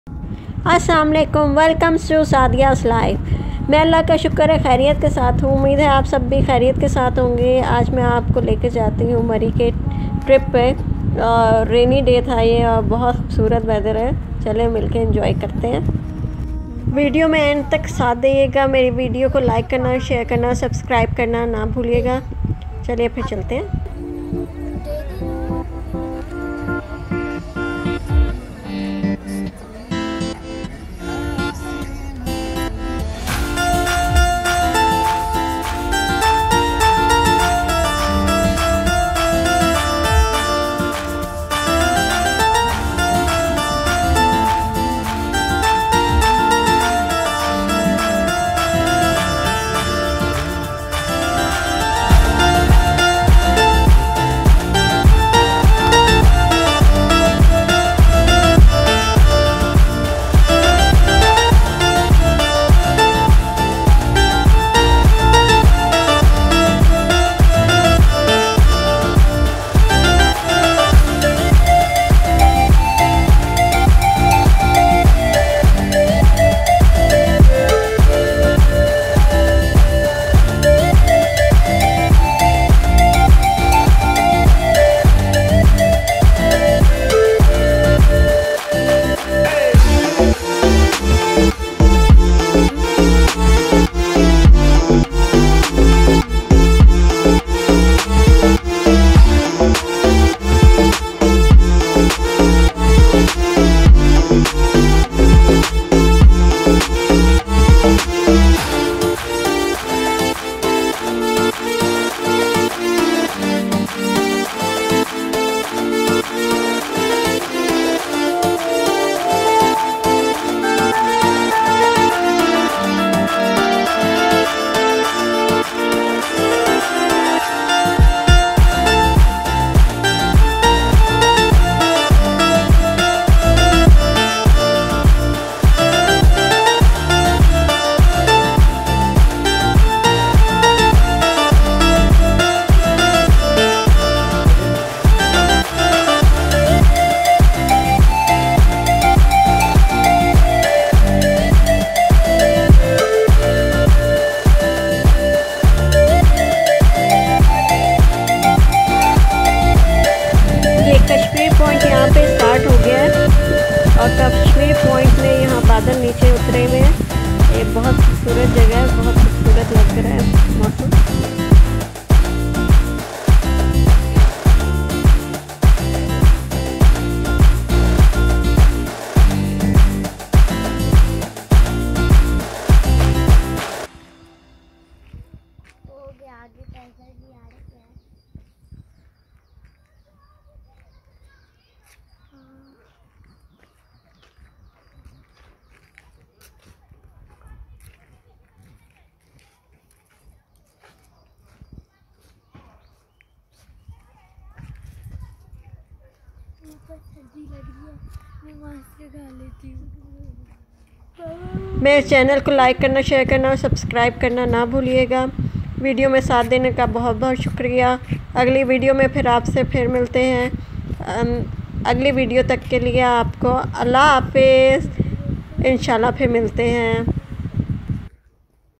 Trip to murree hills in rainy day
Road trip to murree hills in rainy day and beautiful weather